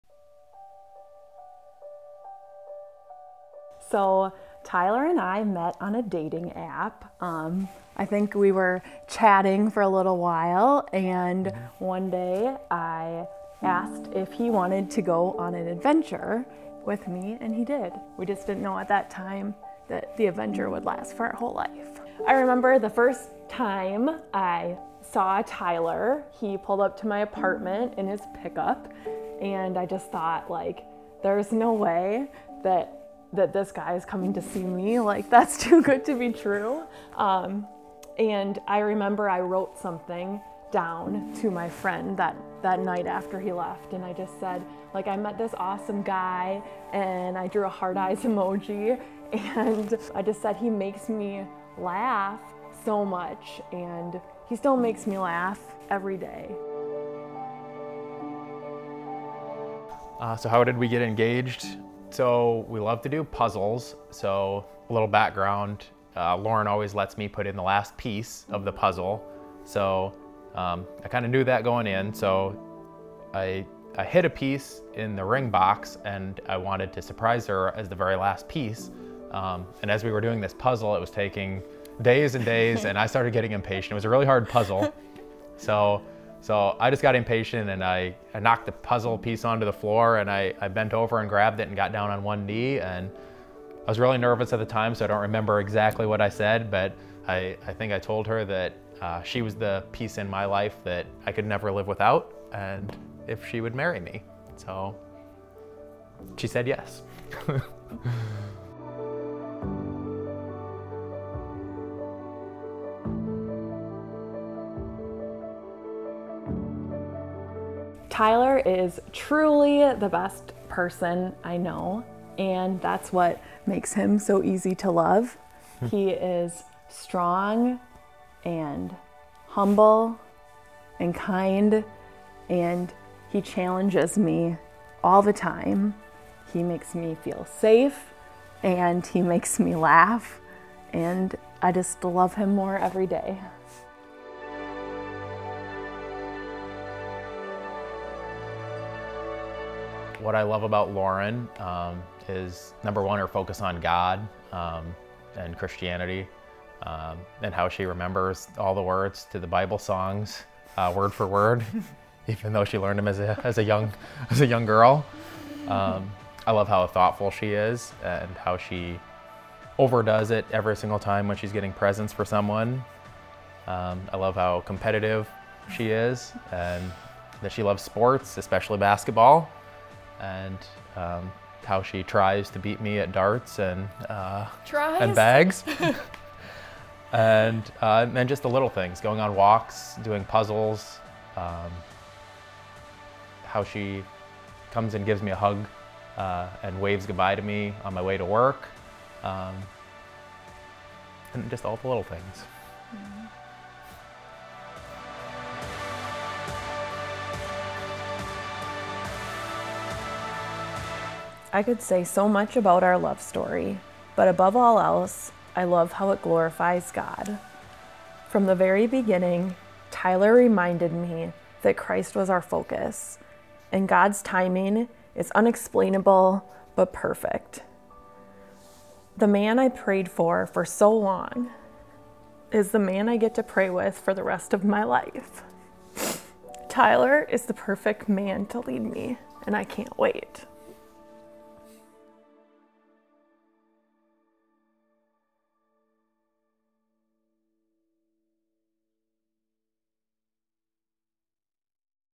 Tyler & Lauren's Love story
lovestory engagementvideo engagementvideocontent love lovestoryvideo wedding engagement We had a lot of fun making this love story video for this couple!